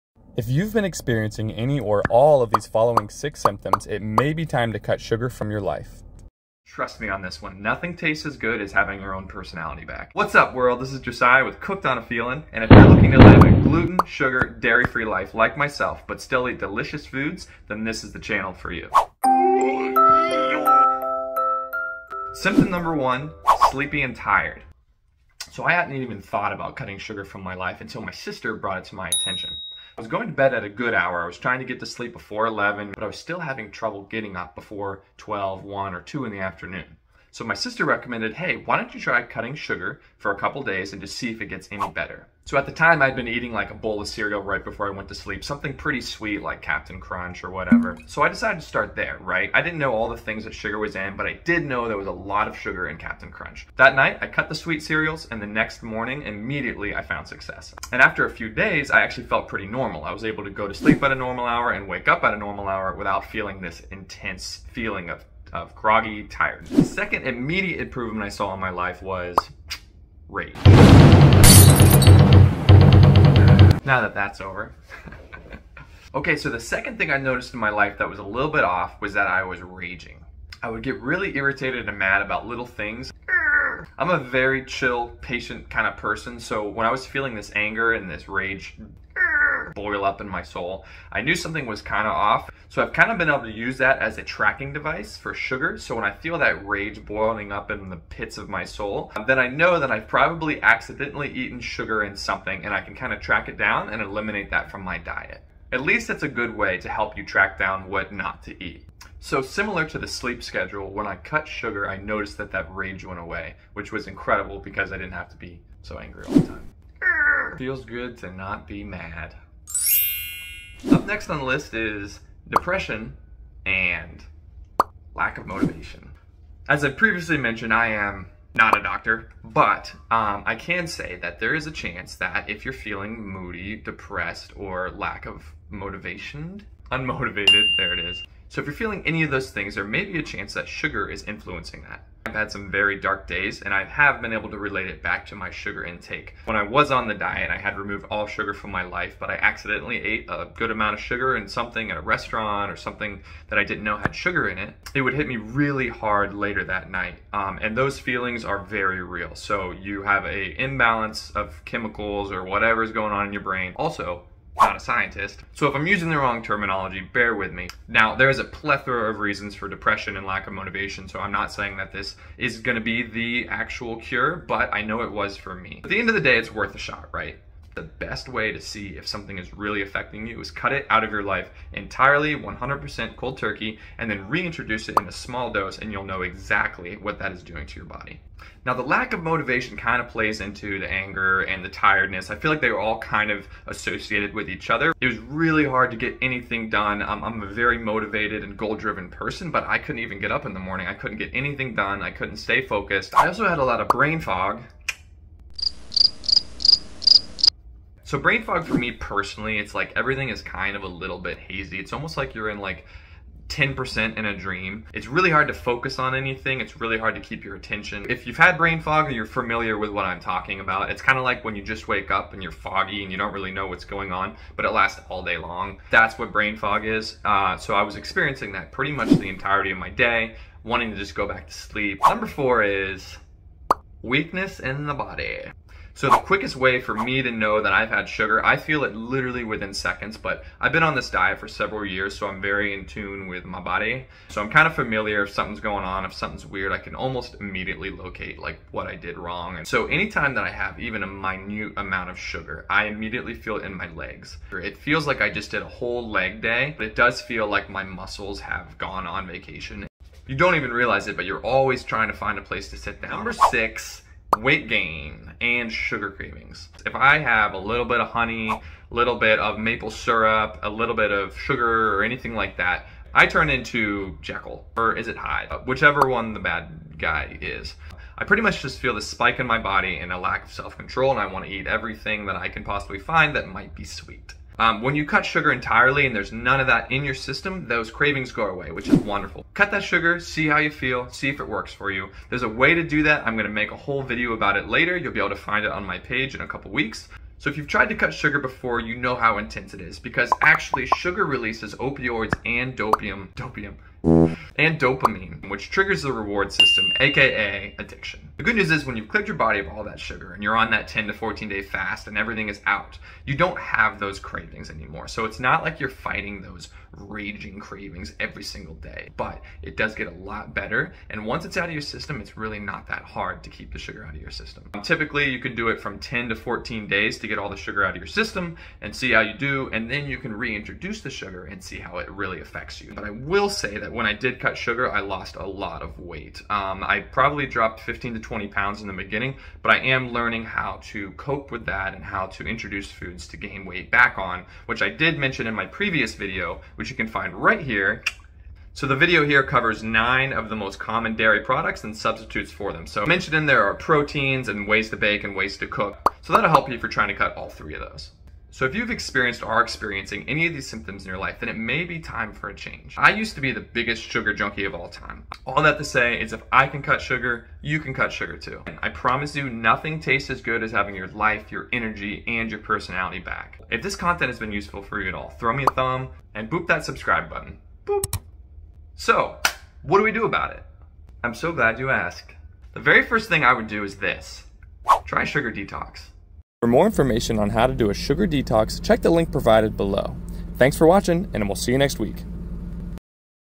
Should I Cut Sugar? Here Are Six Signs Why You SHOULD
Here are 6 signs your body might be using to tell you to cut sugar!

1. 0:23 - Sleepy and tired
2. 1:09 - Rage/Anger
3. 2:01 - Depressed/Lack of Motivation
4. 3:23 - Brain Fog
5. 3:51 - Physical Weakness
6. 4:22 - Weight Gain